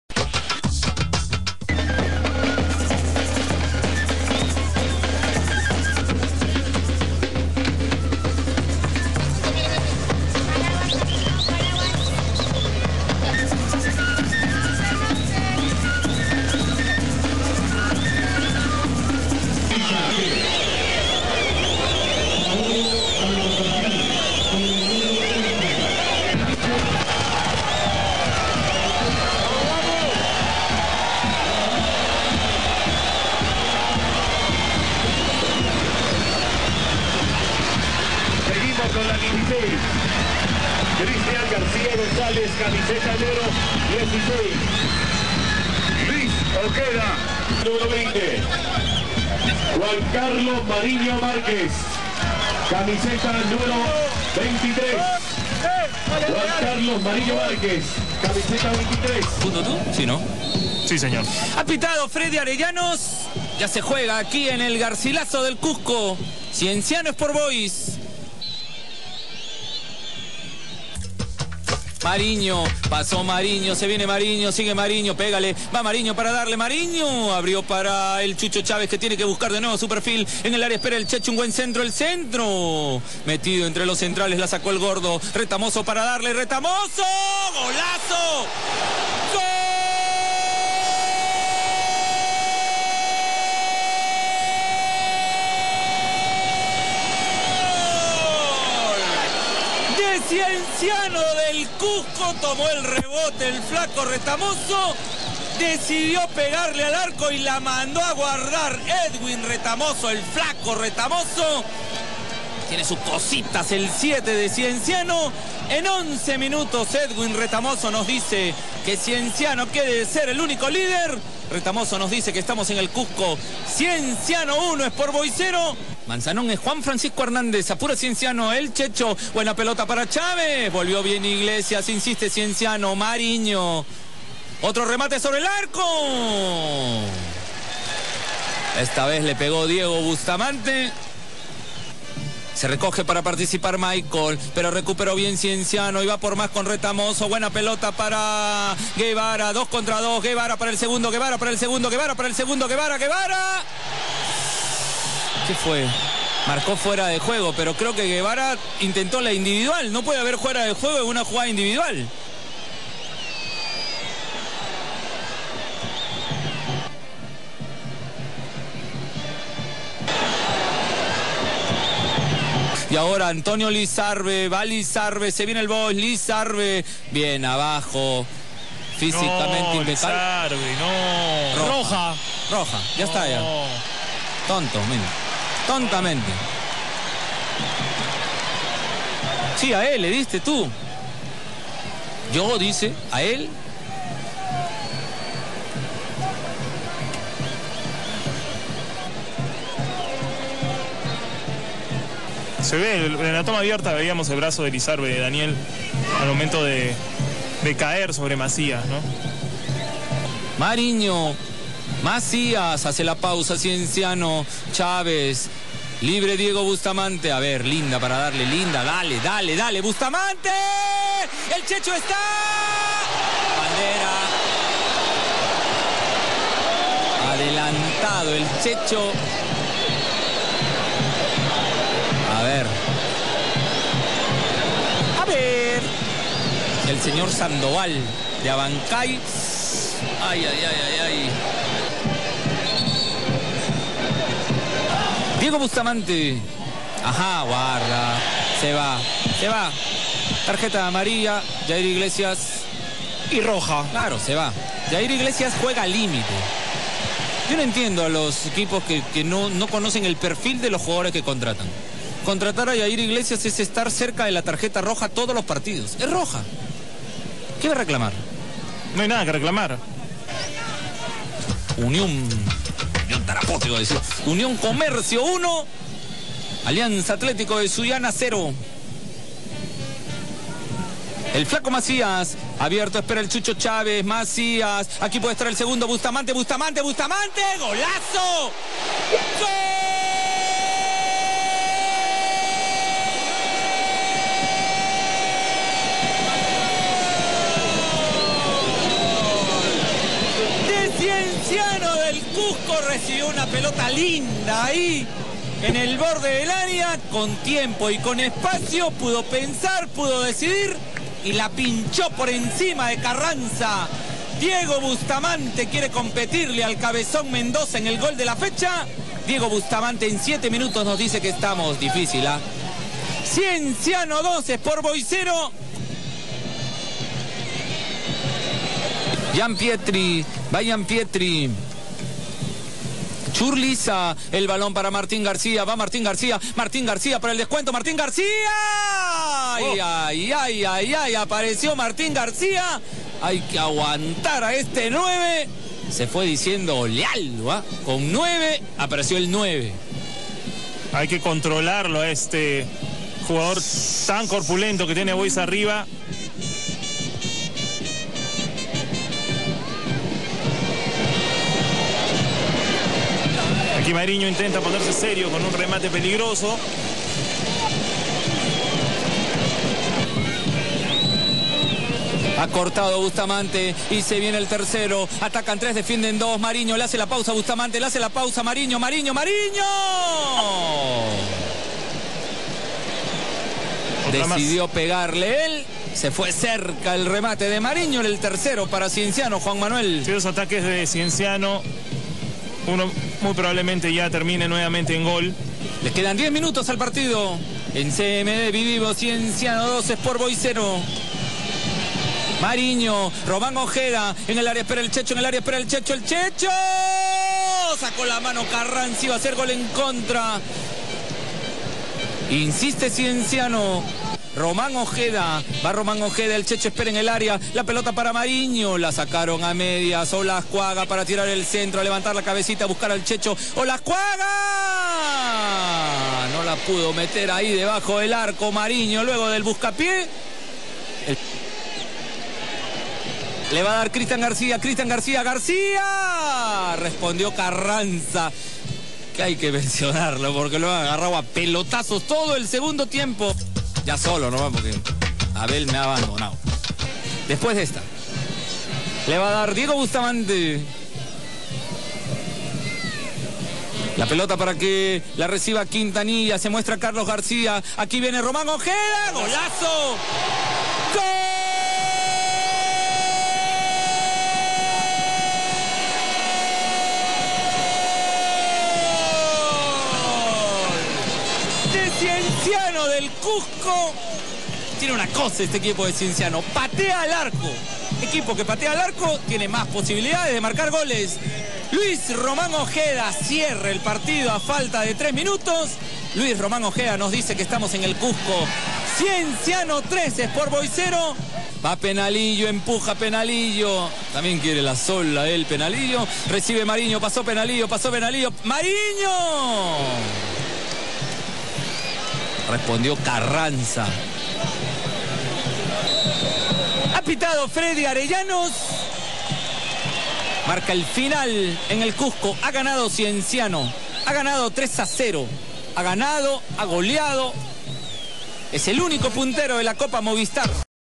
Cienciano 3 vs 0 Sport Boys :: VERSUS
Cienciano 3 vs 0 Sport Boys Resumen Versus - CMD Copa Movistar 2011 Fecha 3